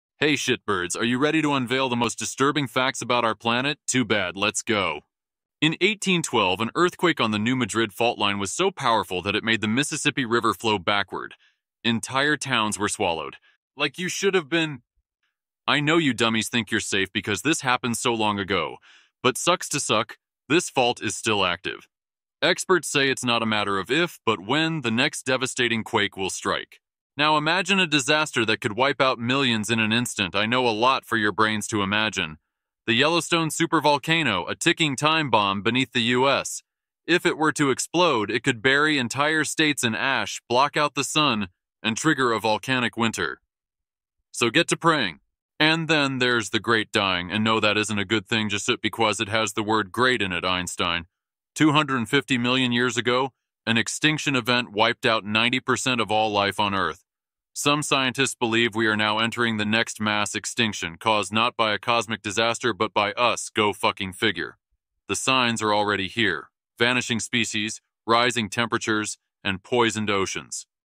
Terrifying Facts About Earth That Will Keep You Up at Night
The Earth may seem like a safe and familiar place, but beneath its surface lies a world of terrifying secrets. From deep-sea death traps to unstoppable natural disasters, our planet is full of chilling mysteries that most people don’t know about.

🔹 The ocean’s deadly brine pools
🔹 A fault line that could make rivers flow backward
🔹 The Yellowstone supervolcano’s catastrophic potential
🔹 A mysterious explosion that flattened an entire forest
🔹 Signs of Earth’s next mass extinction

These are not just stories… they are real, and they could happen again. Watch until the end to uncover the dark side of our planet.